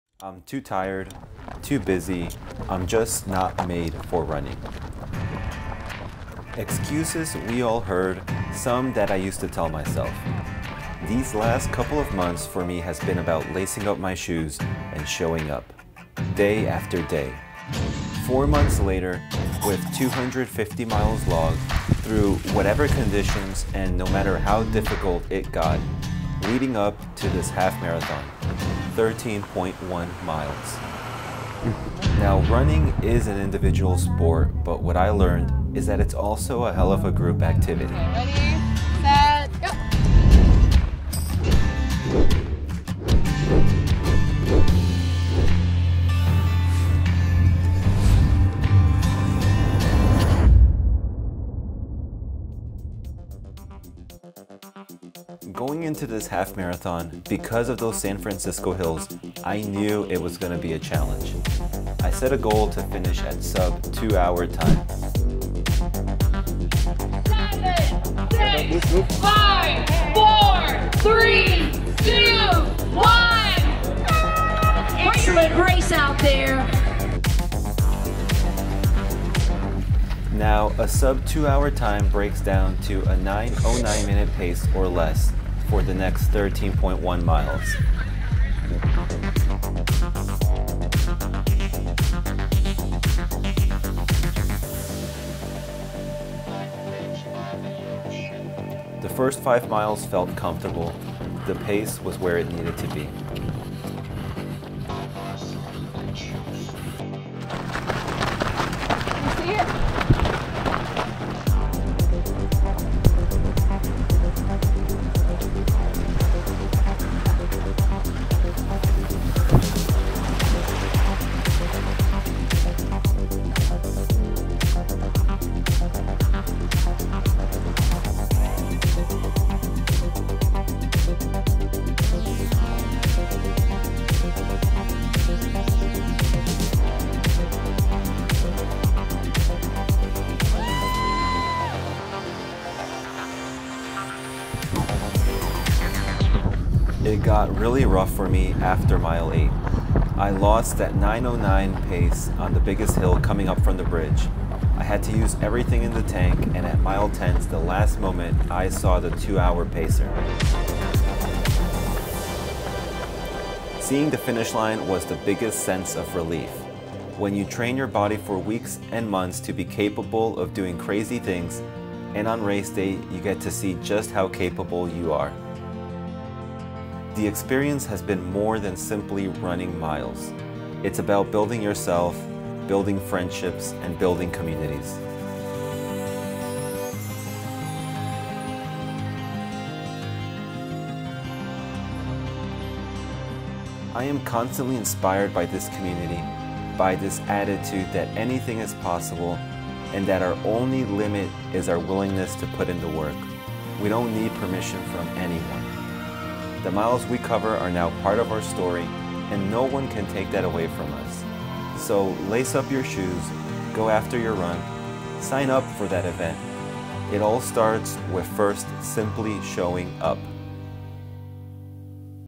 San Francisco Half Marathon Experience 2022!
The story of signing up for a half marathon (July 24, 2022) and committing to my training.

Join me in a run if your in the Long Beach(ish) area:
Social Hour Club: